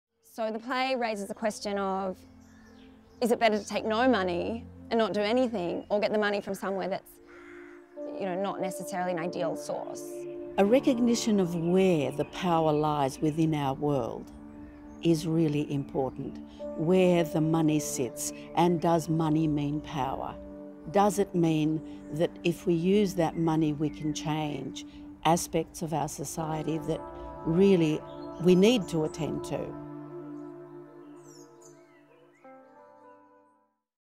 Ngaire Dawn Fair, Actor and Nadia Tass, Director Talk About Extinction
Extinction is a timely and intelligent new Australian play by Helpmann award winning playwright and screenwriter Hannie Rayson (Hotel Sorrento, Life After George), directed by acclaimed film director and producer Nadia Tass (Malcolm, Matching Jack, Disgraced). It delves deep into the heart of our own morals, choices and tightly-held convictions. Extinction wraps an important conservation message around a unique and personal human story. What would you choose?

The tiger quoll once ruled the dense Otway forest but is now almost extinct. A wild, rainy night, a twist of fate and an injured tiger quoll bring together a passionate environmentalist and an unlikely Good Samaritan. Both are hell-bent on saving the species, but intentions are murky. What will be compromised in the quest to save the quoll? Nothing is black and white in this intriguing story about love, sex, money and power played out under the shadow of global warming.